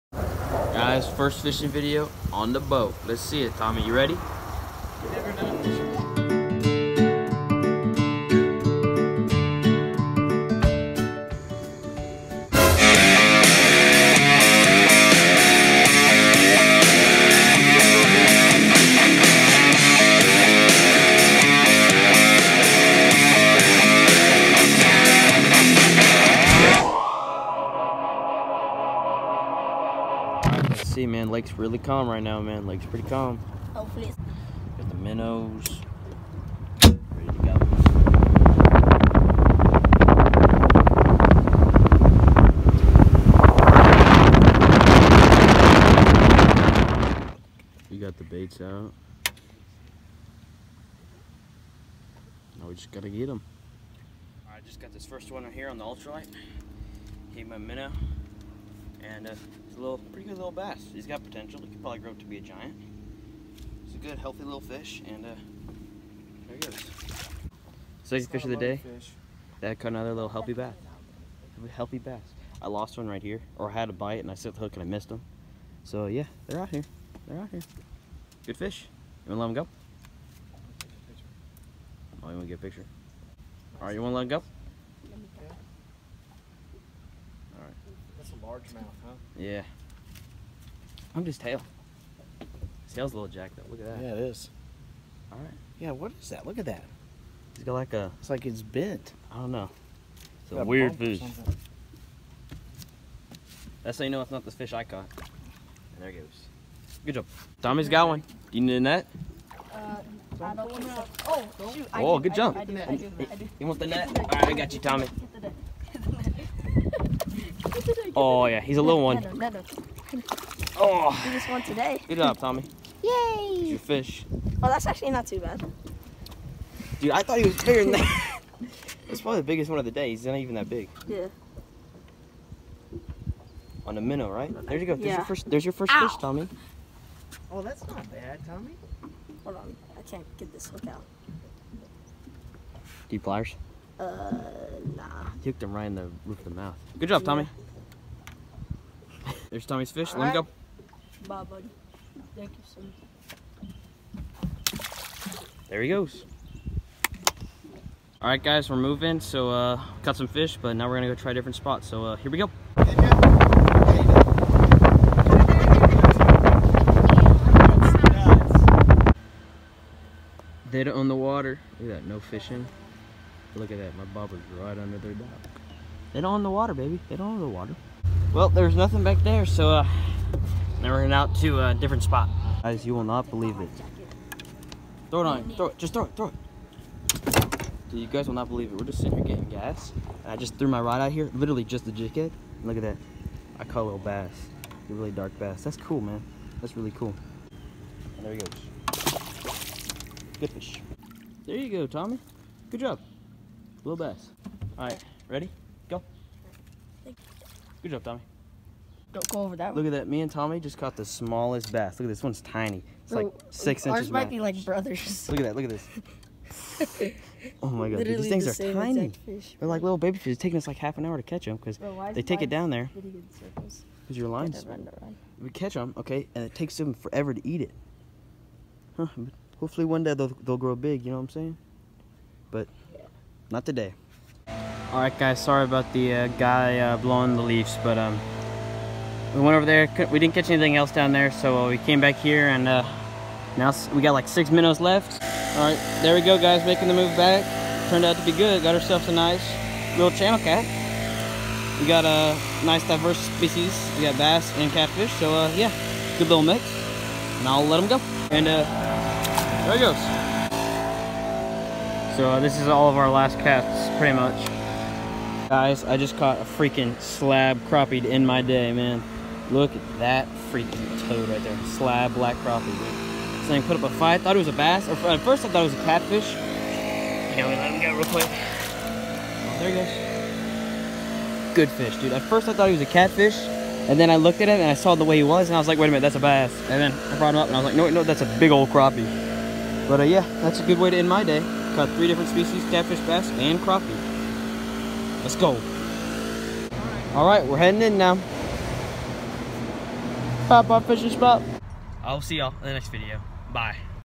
Fishing with Minnows on Lake Conroe!!!
Good day out on lake conroe we had some fun fishing with minnows caught lots of little ones but couldn’t get any size nonetheless we had a great day out on the water.